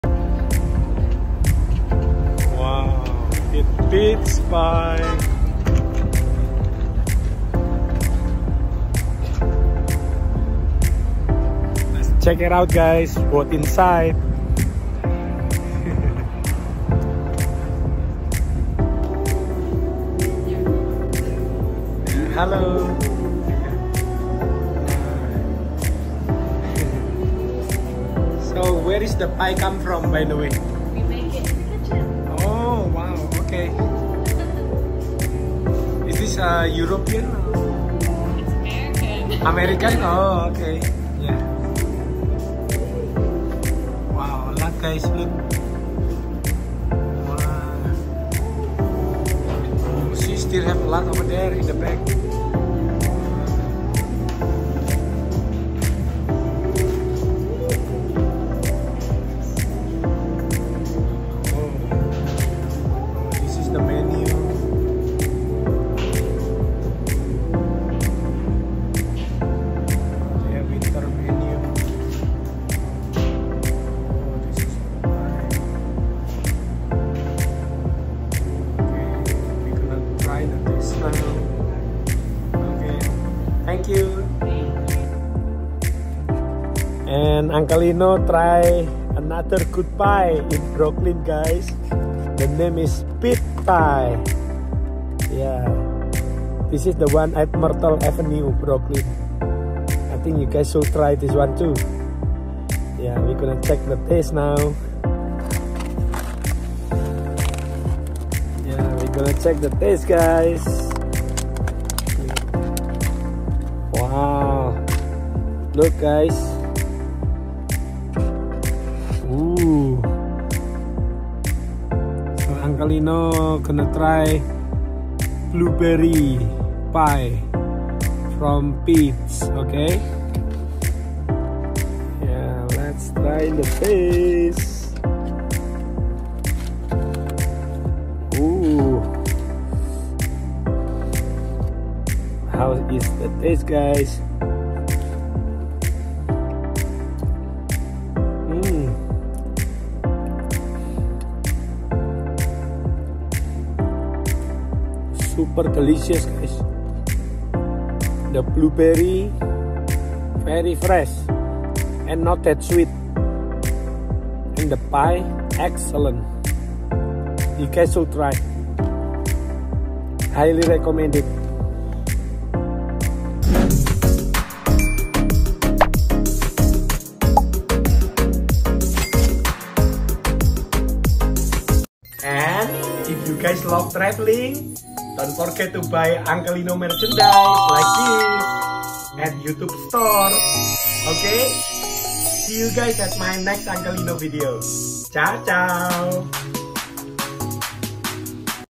PETEE’S CAFE || The King Of Pie
Let’s check The King Of Pie, The one at Brooklyn, New York guys 😋🥧